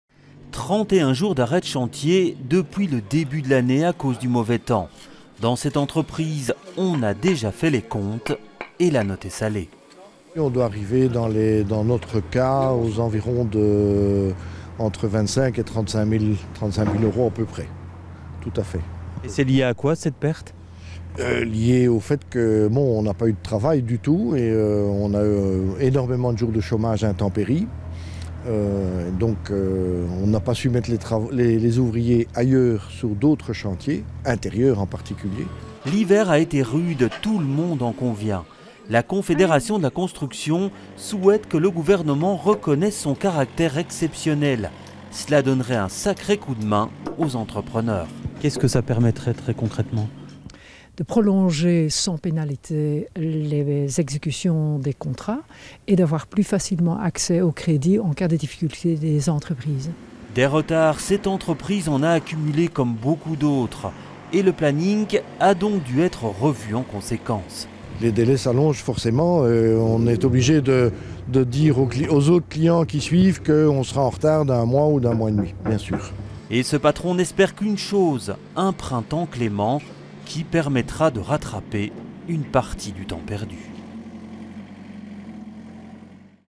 La construction souffre des rudesses de l'hiver
L'hiver est rude cet année. La fédération belge de la construction demande que l'on qualifie cet hiver "d'exceptionnel", ce qui permettrait de prolonger les éxécutions de contrats et surtout d'éviter les amendes en cas de retard.